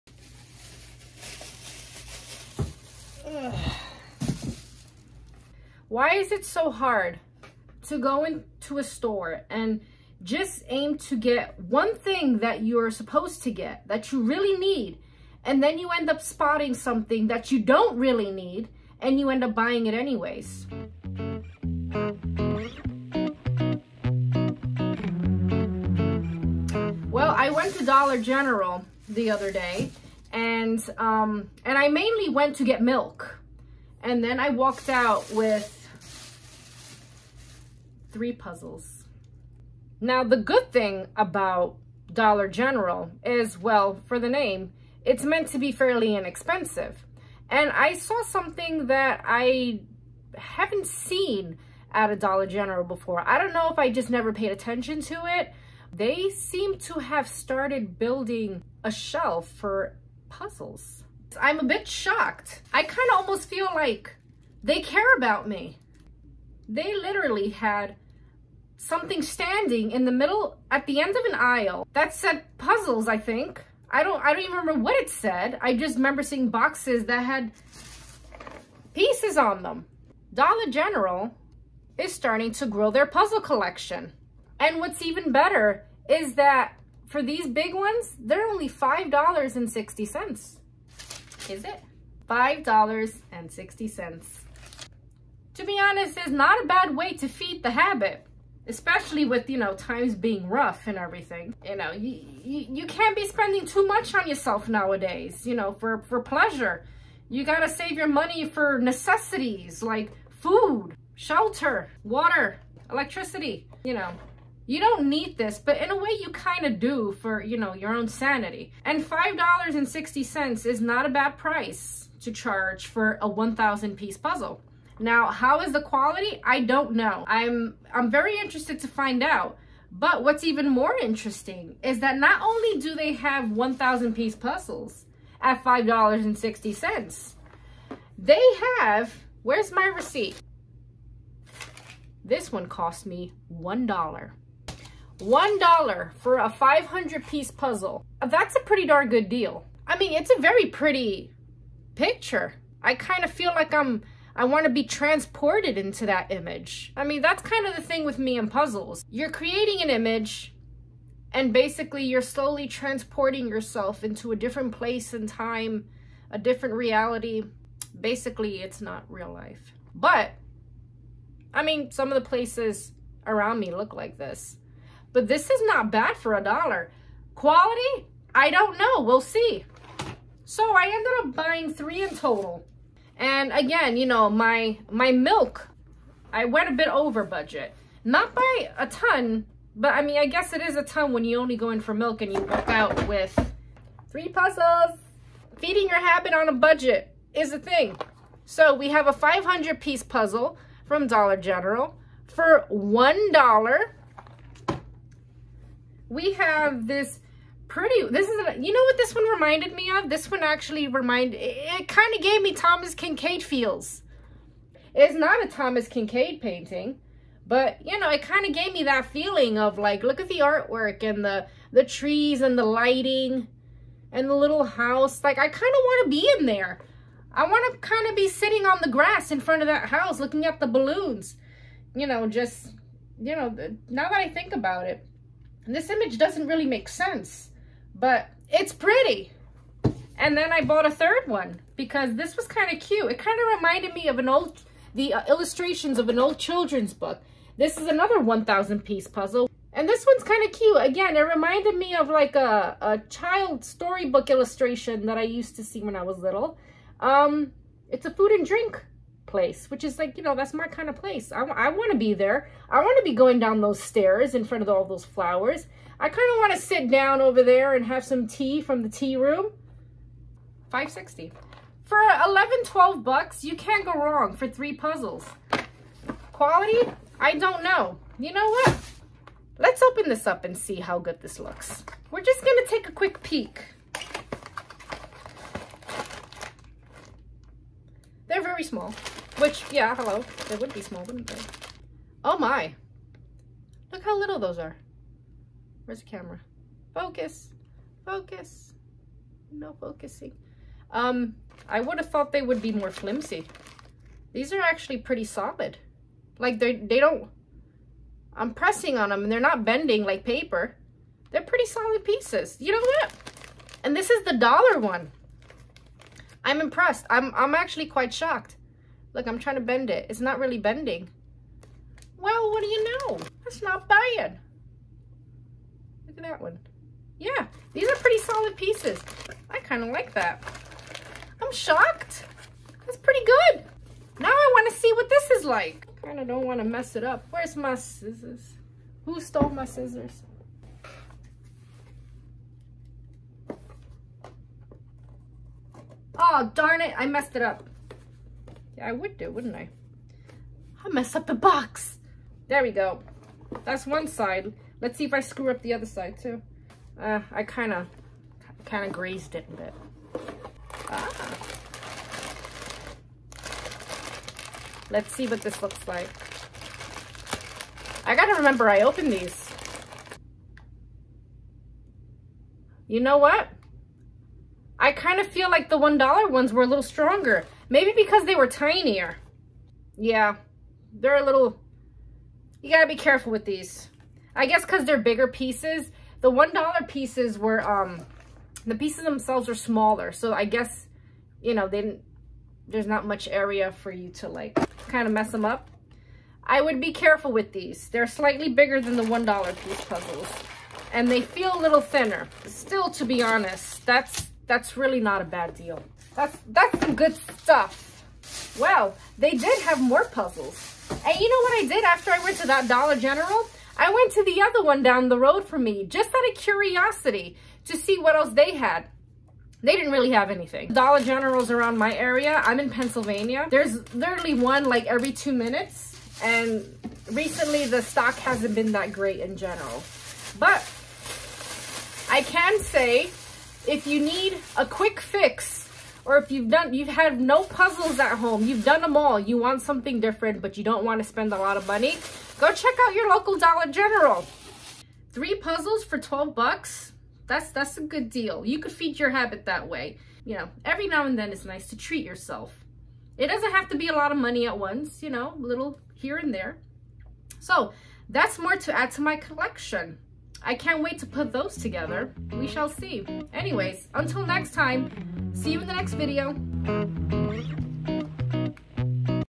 How I Feed My Puzzle Addiction on a Budget: Dollar General Surprises
(Sorry for the video quality! This was before I switched rooms and camera settings.)
Well one of my "quick" trips to Dollar General didn't go quite as planned .  I went in with the intention of only buying milk, but I came across an interesting find while walking down the toy aisle with my daughter.  Seems like Dollar General is expanding their puzzle selection, and I'm all for it!  Here's to hoping the store shelf is kept well stocked...but that might not be too good for my budget if there are too many to choose from.

Mailbox:
The Puzzled Mandy
Lake Harmony, PA 18624

0:00 A Puzzling Find
2:20 The Unexpected Budget Haul
4:35 Quality Check (500 piece)
5:30 Quality Check (1000 piece)
6:42 Final Thoughts